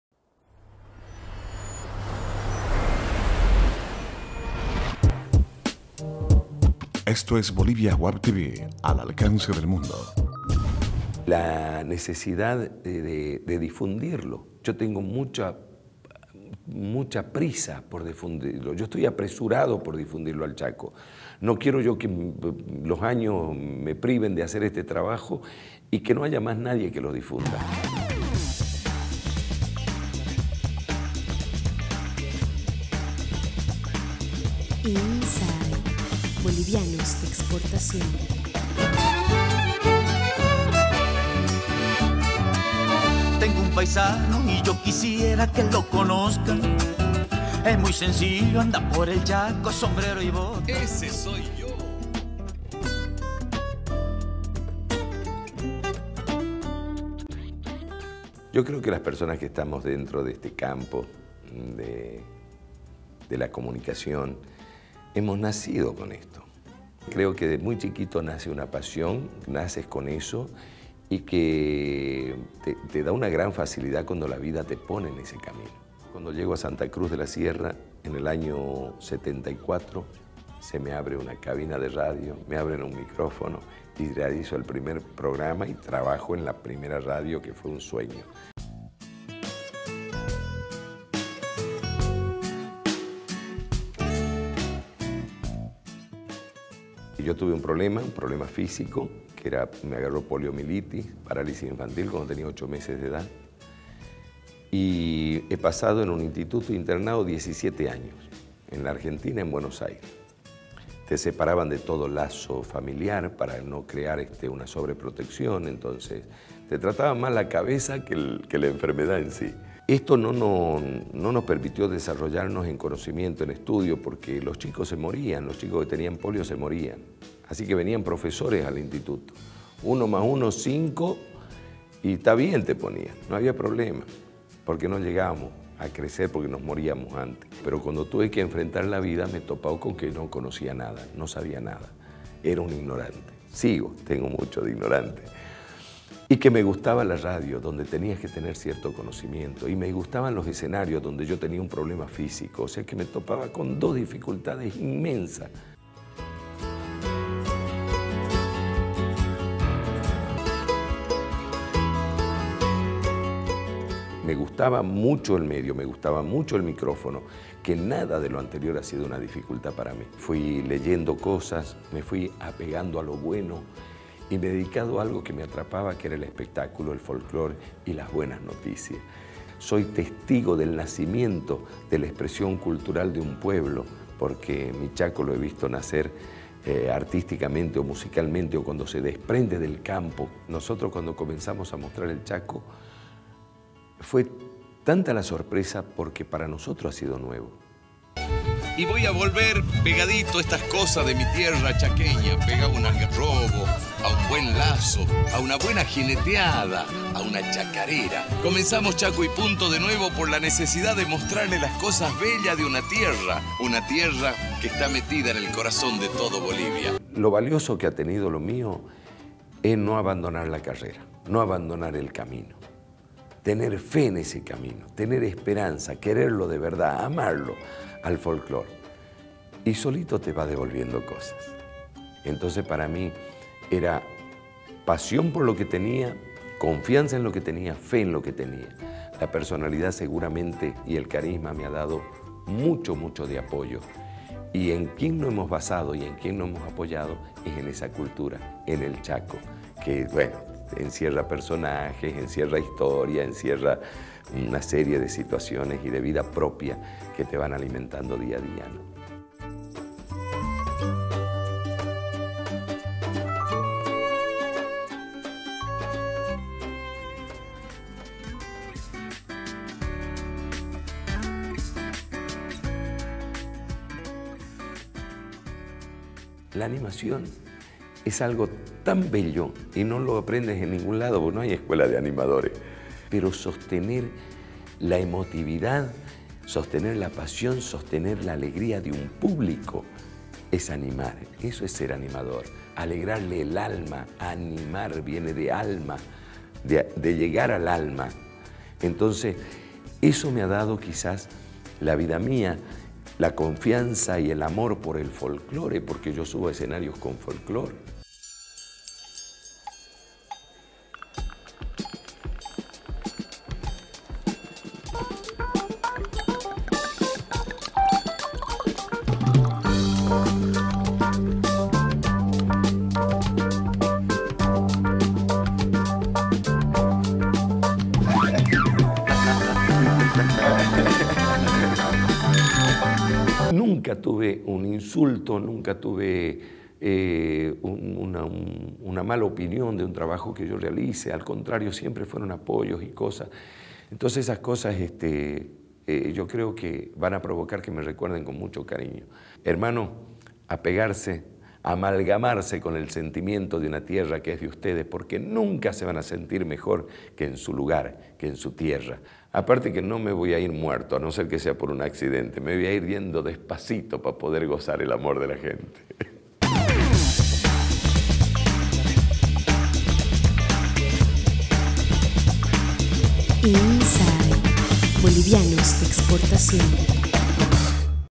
INSIDE Carlos Langa
En esta edición de INSIDE conoceremos al animador, presentador y locutor Chaqueño Carlos Langa.

El boliviano Carlos Langa lleva más de 35 años de carrera en el mundo del folklore. Crea el programa  “Chaco y Punto” en el año 1998, con el que continúa hasta ahora promoviendo la cultura del chaco boliviano. Langa además de ser locutor de radio y presentador de televisión, es animador de el escenario de una de las ferias más importantes de Bolivia, Expocruz, desde hace más de 10 años. .